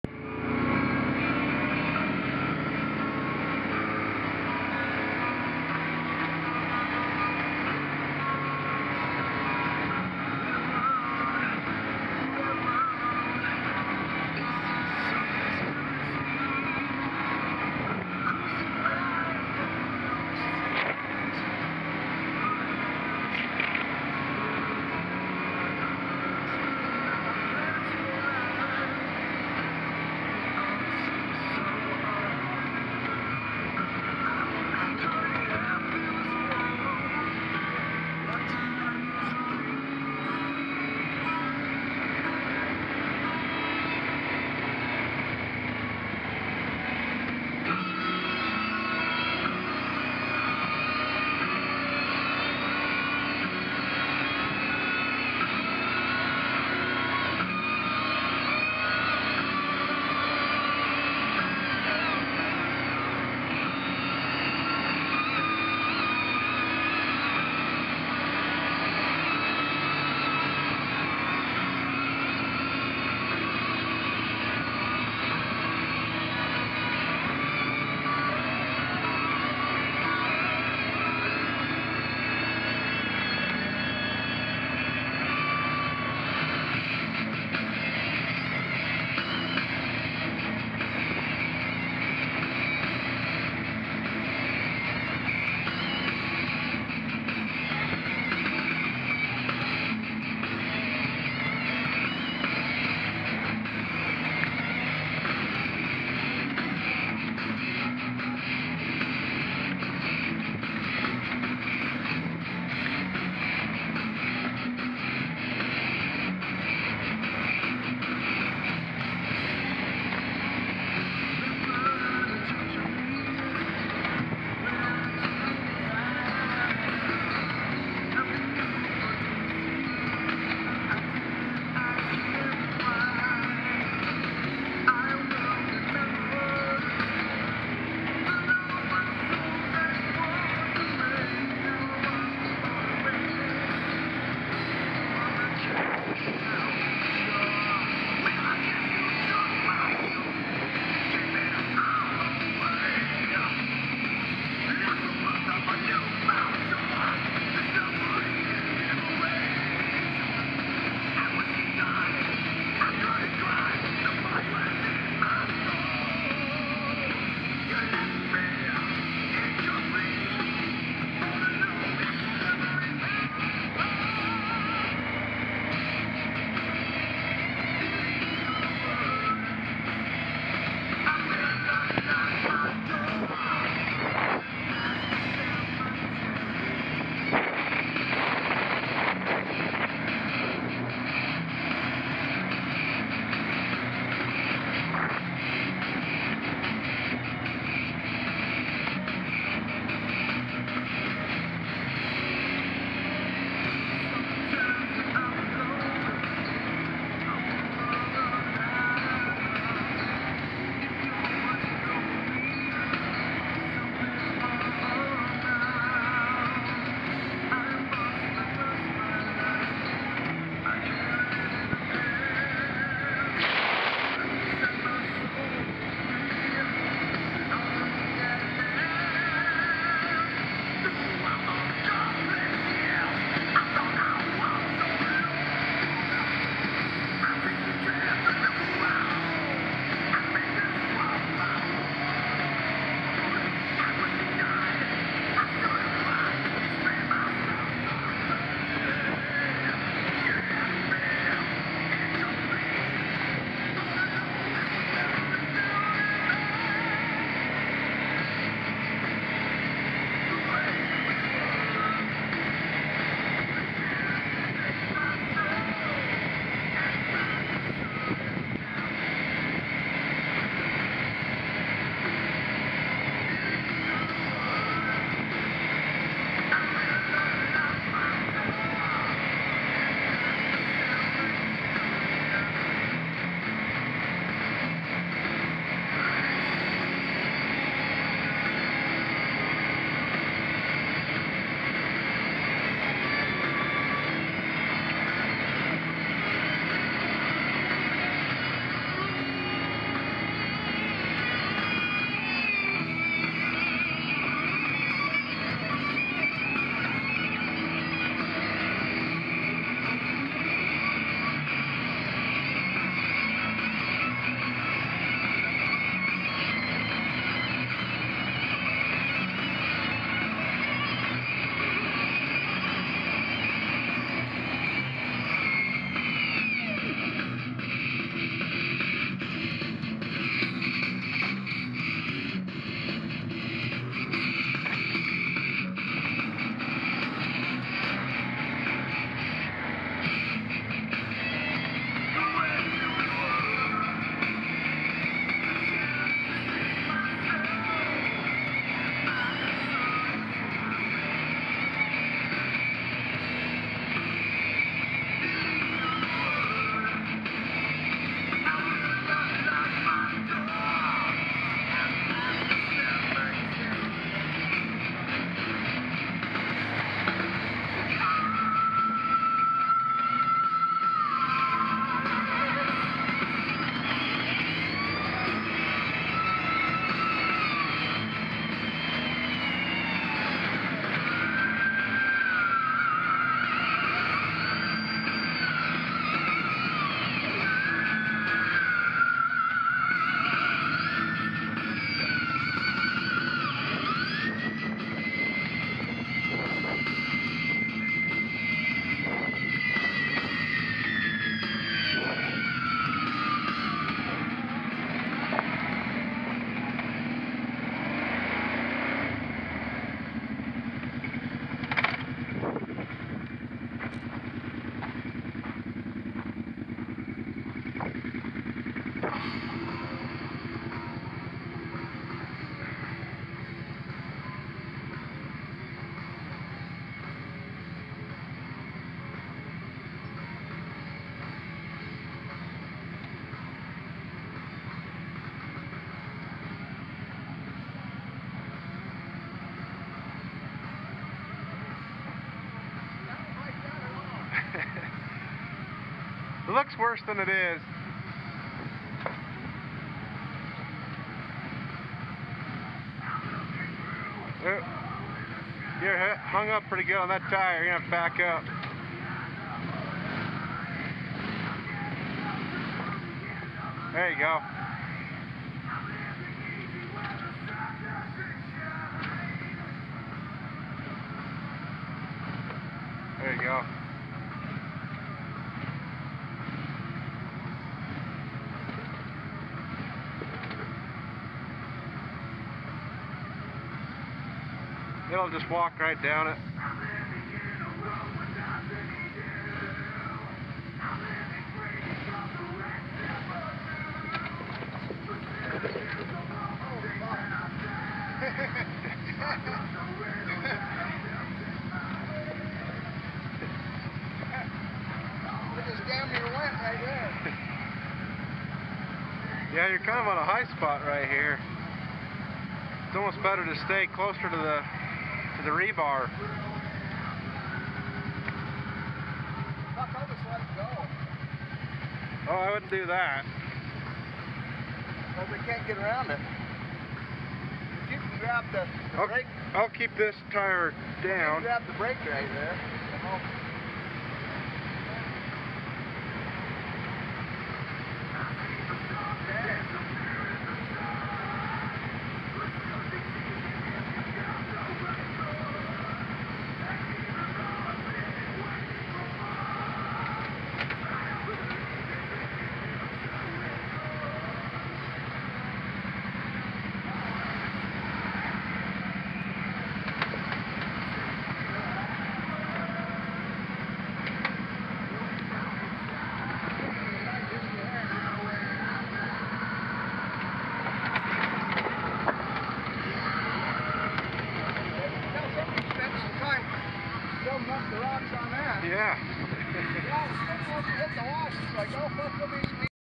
Behind the Reef....A tight spot at Cistern Canyon
This is a clip of some of the 52" section on the Behind the Reef trail in the south San Rafael Swell in Utah.  The last few minutes are where it drops into Cistern Canyon and becomes a bit of a "squeeze".
Caution:  This video contains adult language and heavy metal music.  If either offends you, don't watch it!